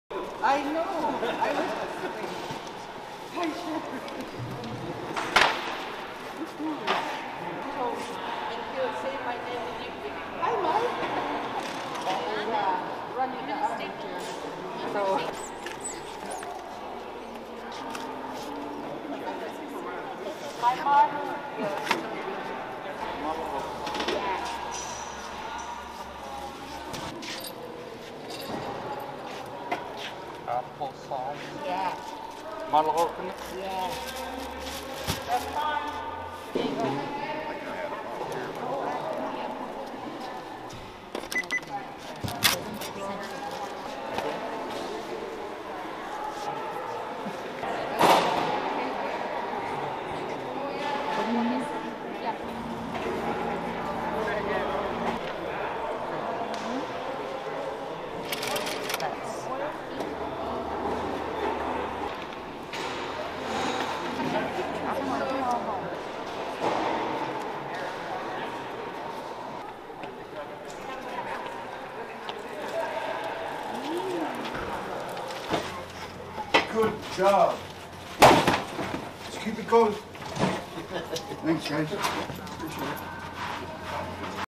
Alaska National Guard assists Commissary in serving rural Alaska
BETHEL, AK, UNITED STATES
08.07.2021

7, 2021, at the Alaska Army National Guard Aviation Facility, Bethel, Alaska. An Alaska Air National Guard C-17 Globemaster III moved tons of groceries and supplies from Joint Base Elmendorf-Richardson to Bethel to offer the goods to eligible residents at discount prices. (Run time: 01:24) (Video by Spc. Grace Nechanicky)

Film Credits: Video by Spc. Grace Nechanicky
Alaska National Guard Public Affairs

commissary
DECA
Alaska National Guard
Bethel
AKNG